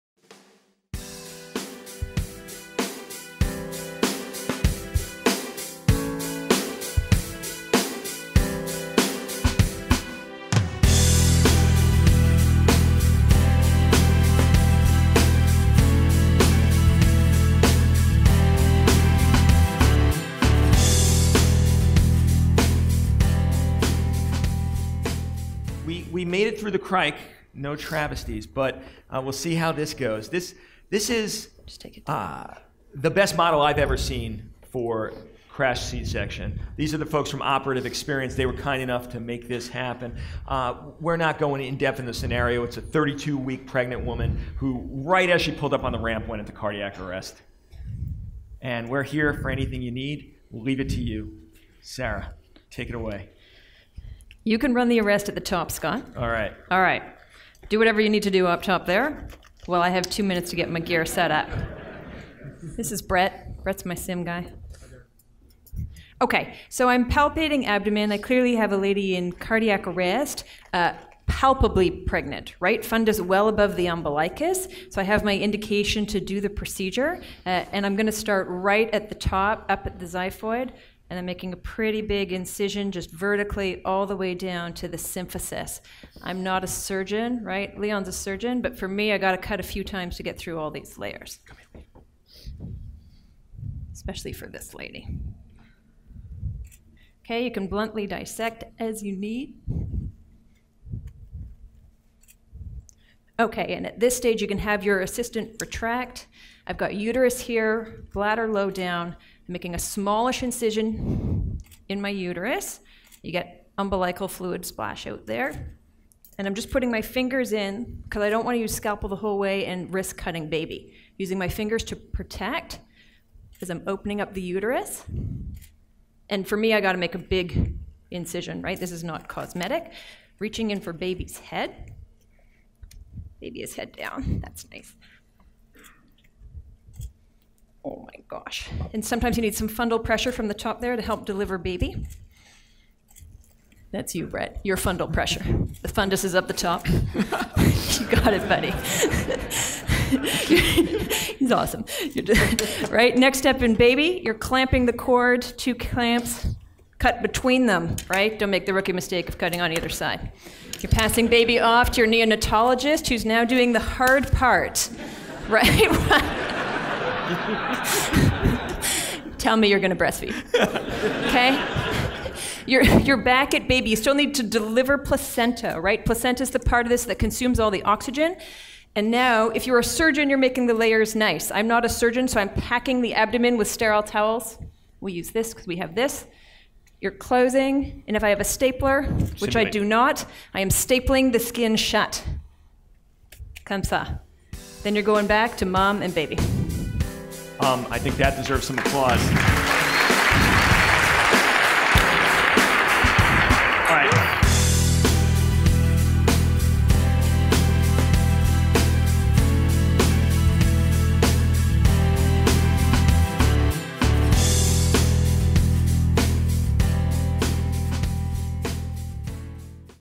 EMCrit Vertical C Section
Demonstration of an emergency vertical c-section using the C-Celia Emergency C-Section Simulator from Operative Experience